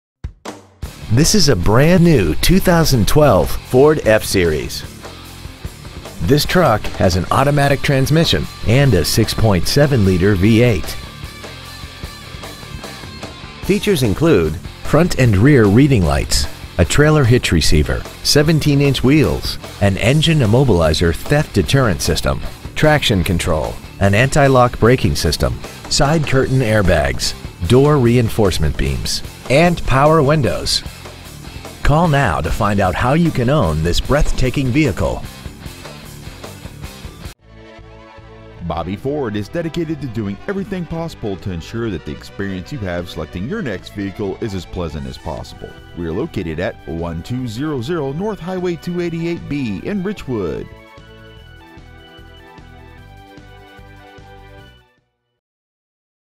2012 Ford Ford F-Series. Richwood TX
We have been honored to serve the Richwood TX area , we promise that your experience at our dealership will exceed your expectations !

 Year : 2012
 Make : Ford
 Model : Ford F-Series.
 Engine : 6.7L V8 32V DDI OHV TURBO DIESEL
 Trans . : 6-SPEED AUTOMATIC
 Exterior : WHITE

 Interior : TAN
 Stock : CEC75987

 Bobby Ford, Inc.